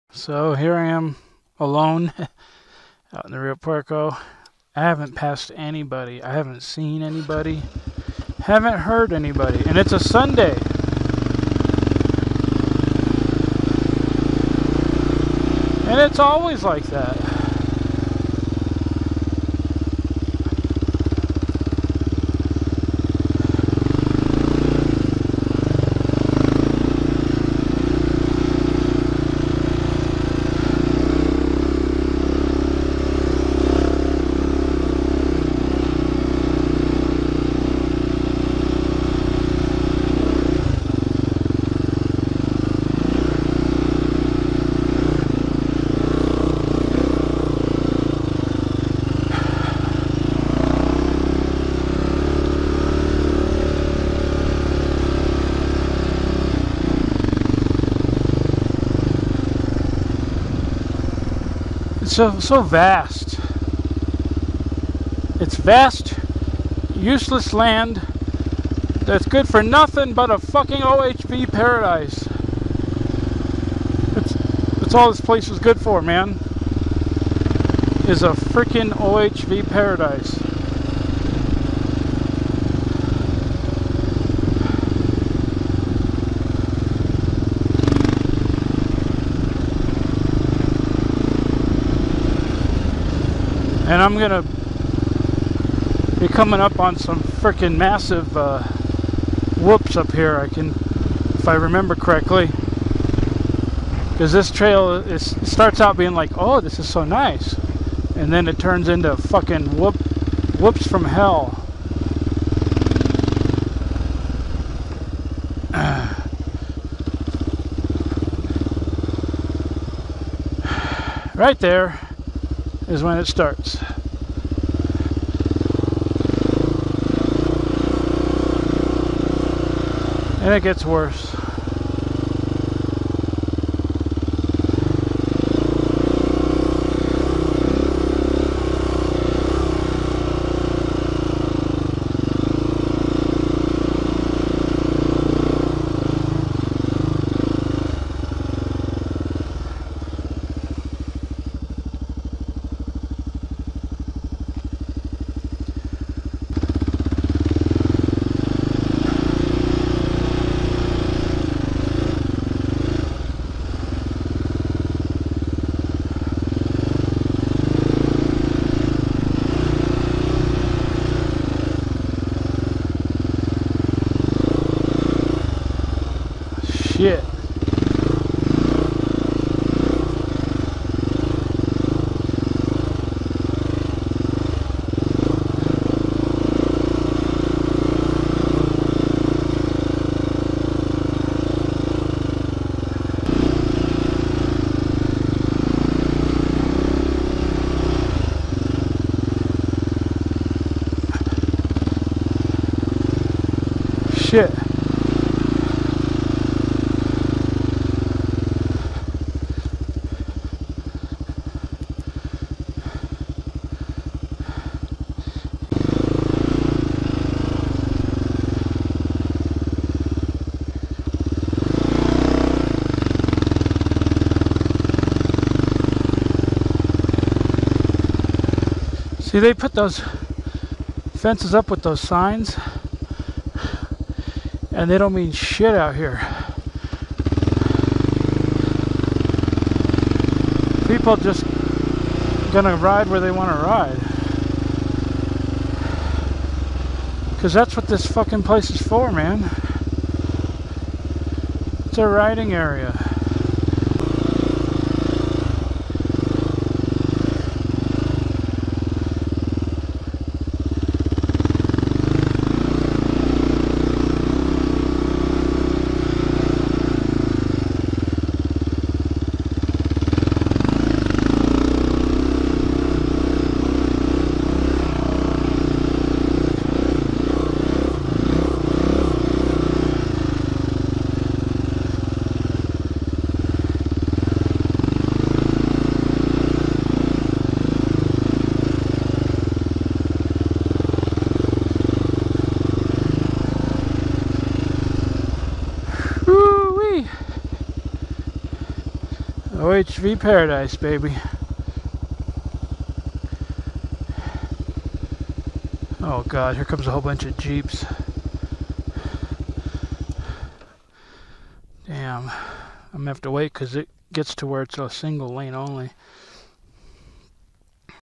KLR Rio Puerco New Trail 2
Continuing along a trail coming from some high cliffs in the Rio Puerco area on my KLR 650.